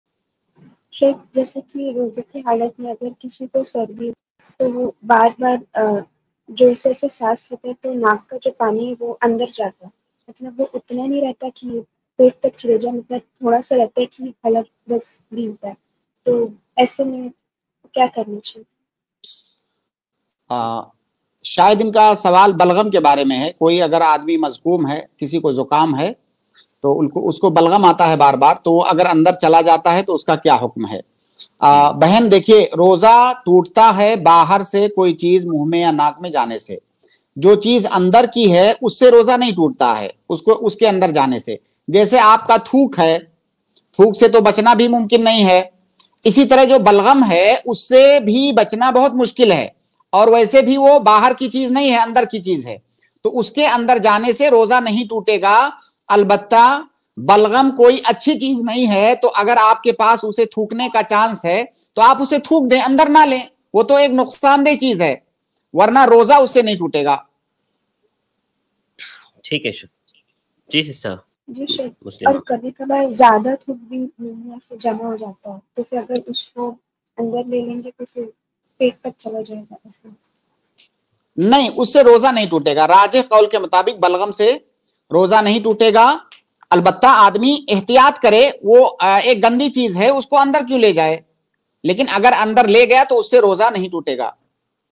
Kya Balgham Aur Thook Nigalne Se Roza Toot Jata Hai Ya Nahi? By Dr. Mohammad Naseem Madani
Kya Balgham Aur Thook Nigalne Se Roza Toot Jata Hai Ya Nahi? By Dr. Mohammad Naseem Madani iPlus TV Deeni Q&A

UPI : donateilm@icici